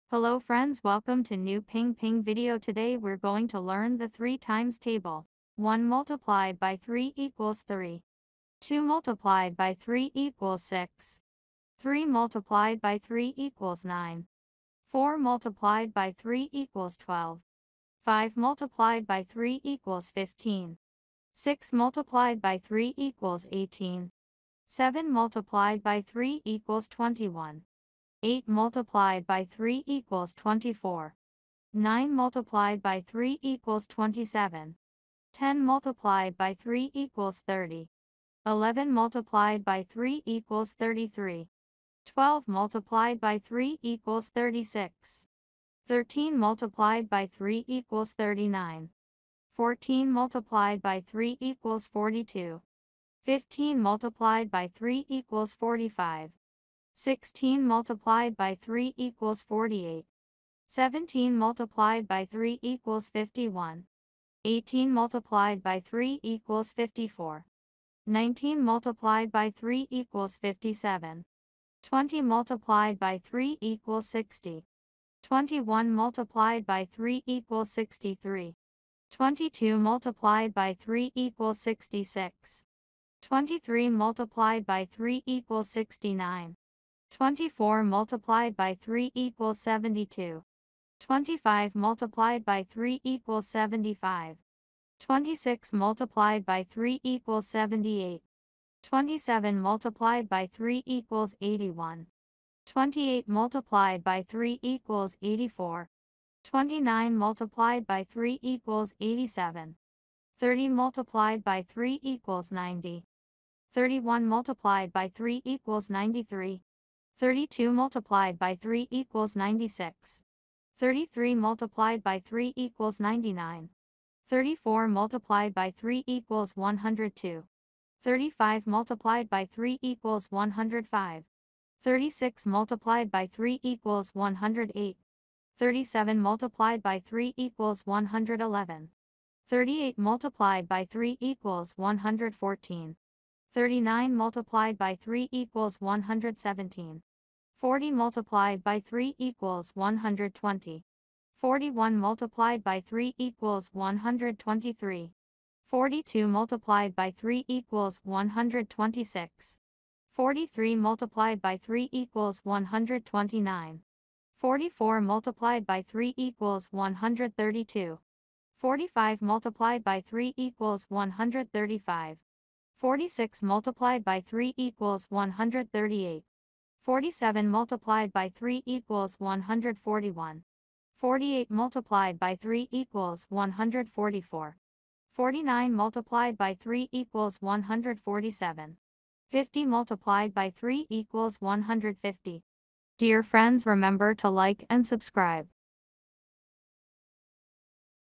Master the 3 Times Table Quickly and Easily with This Fun and Engaging Video!
Grasp the 3 times table effortlessly with this engaging and interactive video tutorial. We'll use catchy songs, colorful visuals, and simple explanations to make learning multiplication a breeze. Whether you're a child just starting out or an adult brushing up on your math skills, this video is for you!